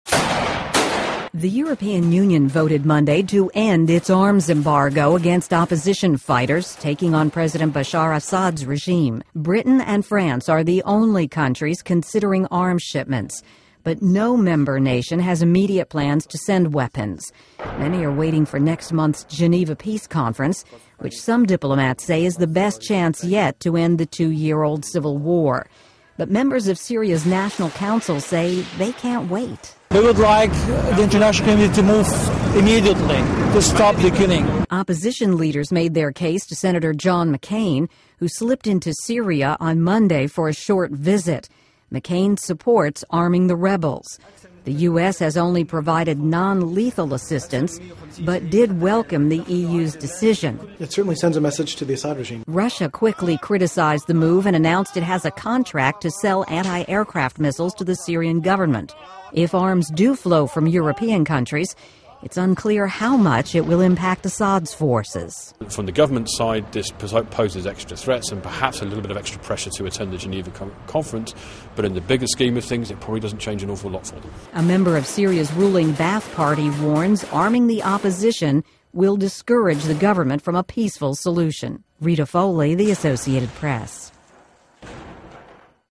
Fears of Foreign-fed Syrian Arms Race
Rebels in Syria may get weapons from European Union countries, but not immediately.  The opposition did get another boost with a visit by Senator John McCain.  (May 28)